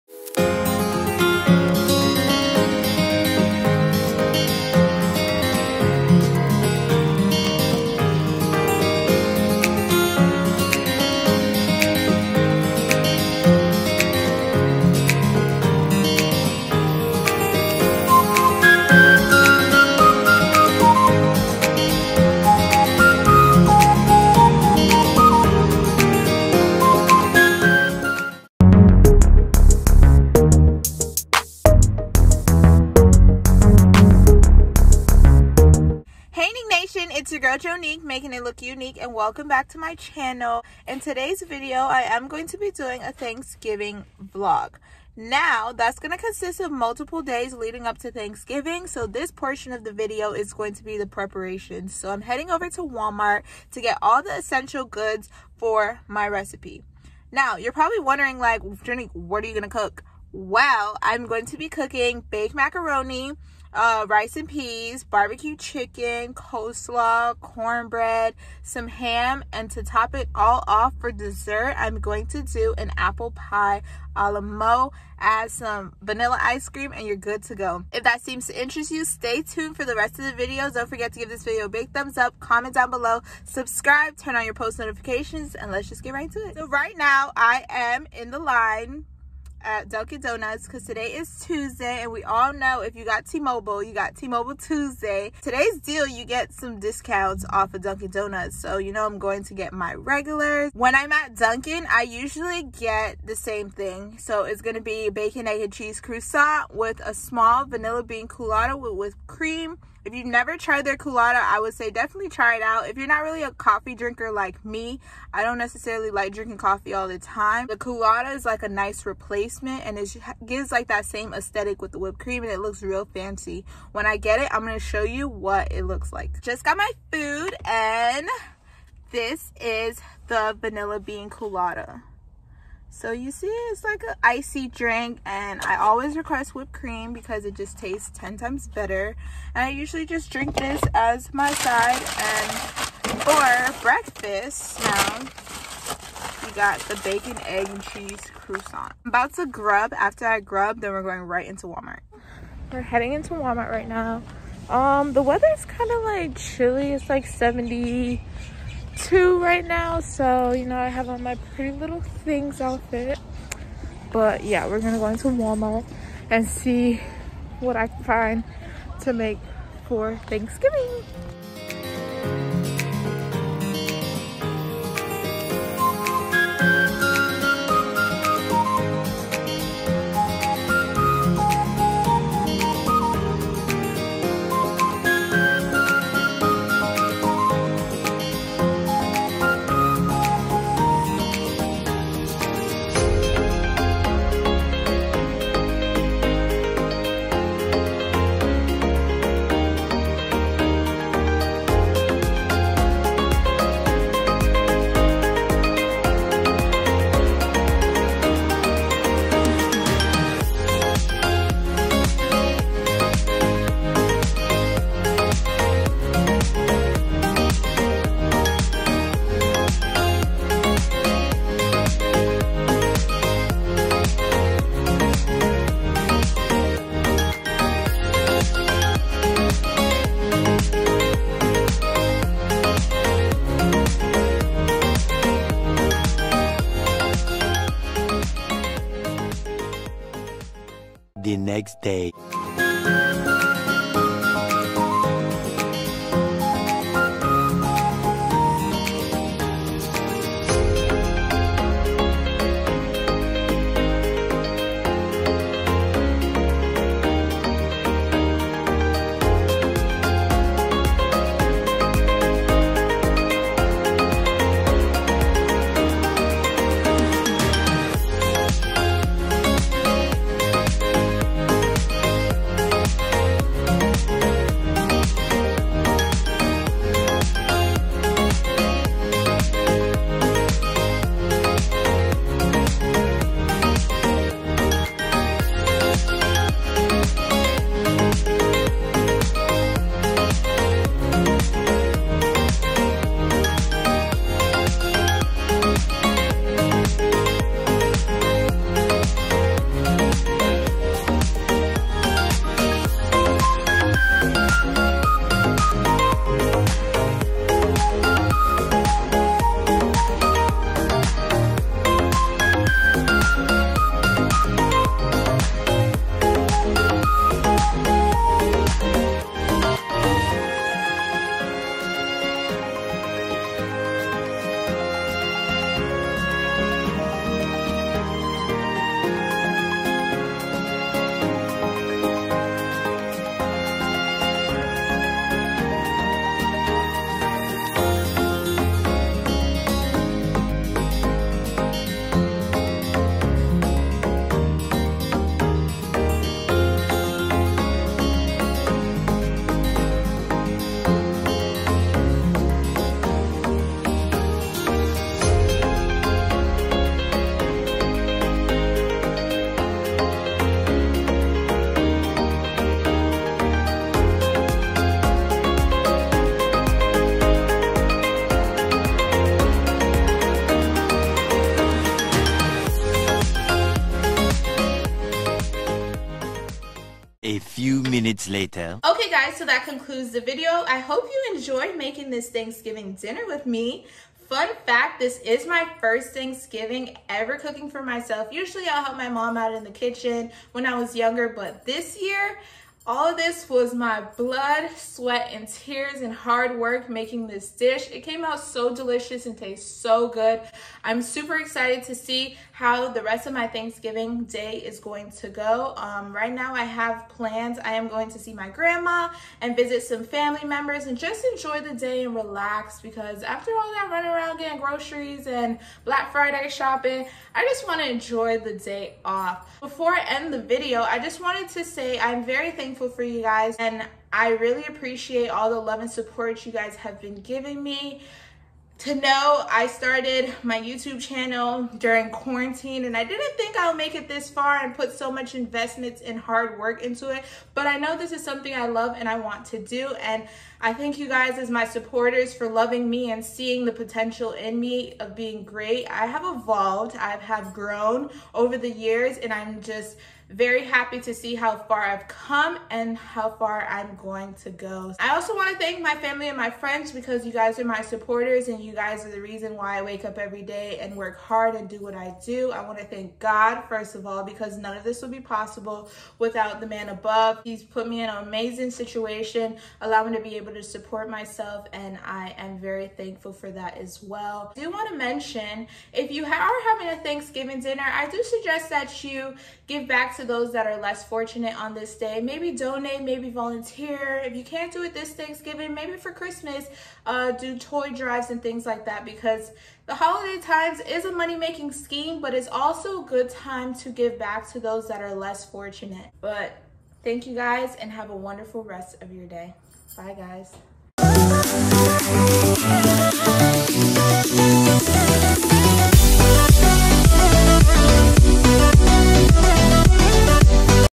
THANKSGIVING VLOG 2021| GROCERY SHOPPING + THANKSGIVING PREP
Hey NiqueNation, Happy Thanksgiving to you and your loved ones. In this video, I will be taking you guys grocery shopping for my thanksgiving meal. You will also see me whipping it up in the kitchen, making items such as baked macaroni, coleslaw, cornbread, bbq chicken, and more. I hope you guys enjoyed this video and happy thanksgiving 🦃🍁🍽.

-FAQ -
Name: Jonique
Age:23

Snap chat: jo.taytay